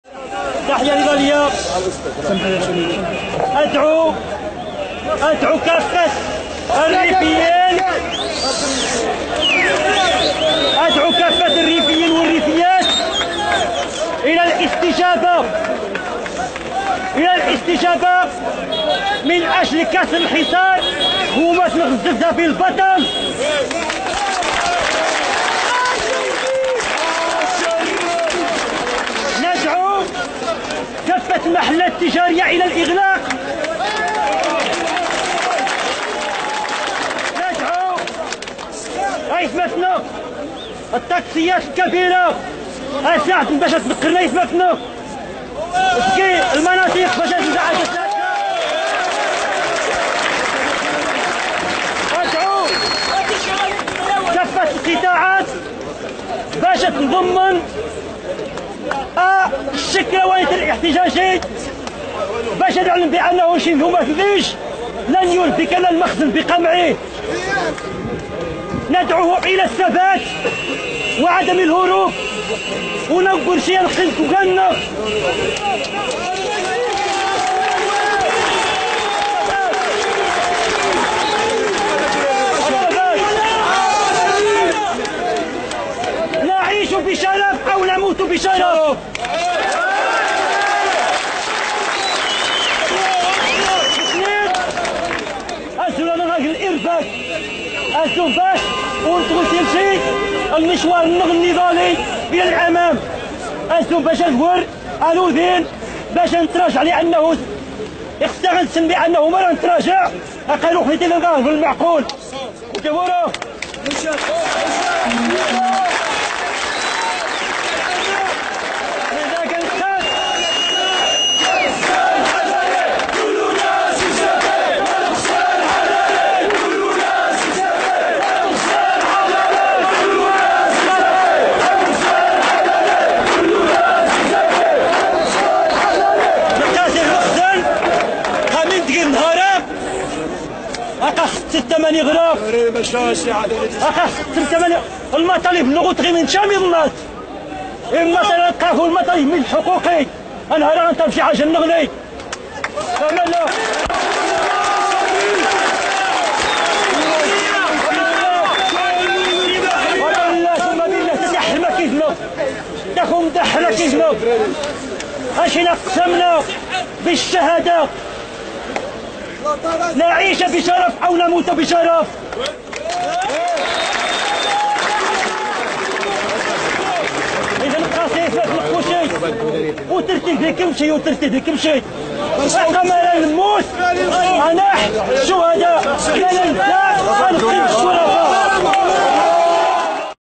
De laatste woorden van Mohamed Jalloul op 26 mei 2017 (RIF/NL)
Mohamed Jalloul (1971) is Riffijnse onderwijzer, mensenrechtenactivist en vakbondsman. Hij zat 5 jaar vast voor zijn deelname aan de 20-Februari-beweging in 2012. Kort na zijn vrijlating werd hij op 26 mei 2017, drie dagen voor zijn medestrijder Nasser Zefzafi, opnieuw gearresteerd voor zijn deelname aan de Riffijnse volksbeweging. Hij is vader van drie kinderen. Zijn minderjarige dochter Houda moest een verklaring afleggen bij de politie nadat zij protesteerde tegen de ontvoering van haar vader.
In juni 2018 kreeg Mohamed Jalloul in een schijnproces 10 jaar gevangenisstraf opgelegd. In hoger beroep van april 2019 bleef de 10 jaar gevangenisstraf gehandhaafd.